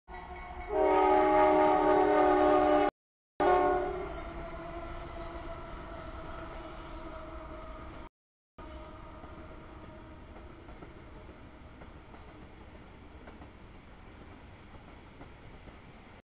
2- 20-2019 05:40 sb tanks CSX 2 BNSF BNSF K048
00:37 sb nysw cut, mix CSX 3065 2 DPU's Q409
00:44 nb stack CSX 5490 L159
01:04 nb stack trailers CSX Q009
01:20 nb stack CSX 3218 L157
02:04 sb stack CSX 743 Q160
03:03 sb tanks UP 4654 K682
04:47 nb auto rack CSX 792 Q261
05:40 sb tanks CSX 2 BNSF BNSF K048
06:09 sb stack trailers CSX 933 Q010
06:47 nb stack trailers CSX 828, 3233 Q003
07:24 nb stack trailers CSX 65, 227 Q007
08:14 sb stack CSX 3192 DPU 3109 Q008
08:32 nb auto rack CSX 5253 Q253
09:53 nb mix, nysw cut CSX 3174, 58, 230, 215, 223, 243, 745 Q410